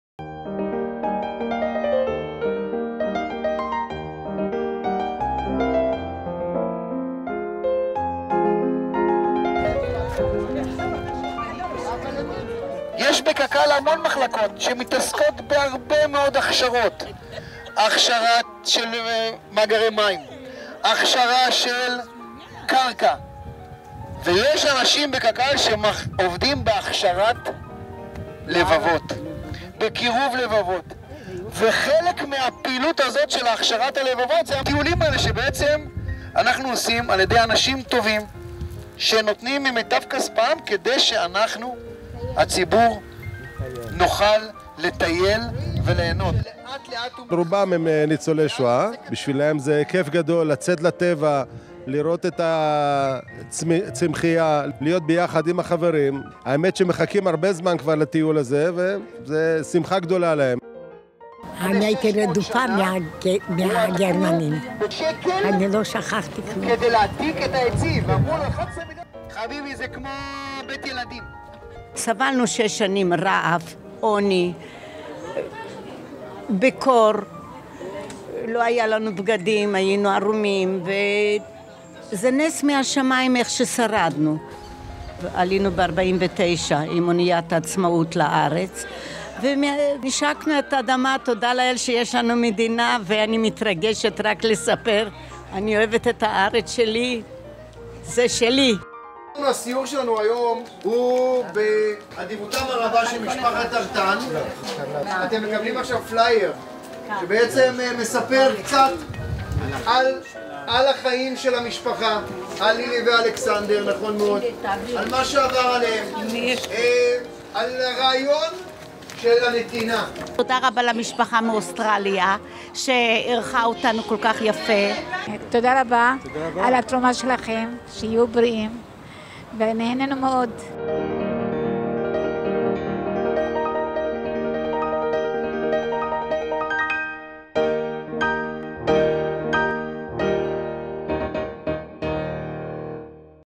Holocaust Survivors Tour the Galilee with KKL-JNF
Holocaust survivors from Haifa’s Kiryat Haim neighborhood embarked on a tour of the Galilee with KKL-JNF on Sunday, April 14, 2019. This day trip was one of many conducted for Holocaust survivors all over Israel, thanks to a generous bequest from the estate of Lilly and Alexander Enten of blessed memory, from Australia.